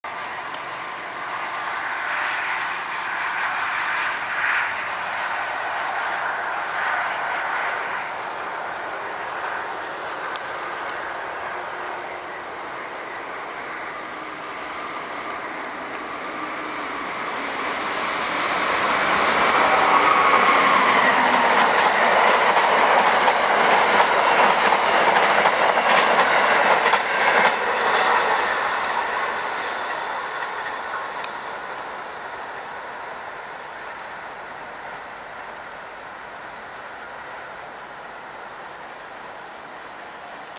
MARC 642 Departs Just In Time
A northbound MARC train departs from the Halethorpe MARC station just as Amtrak Northeast Regional 127 rushes towards Washington D.C. Filmed on 8/1/16.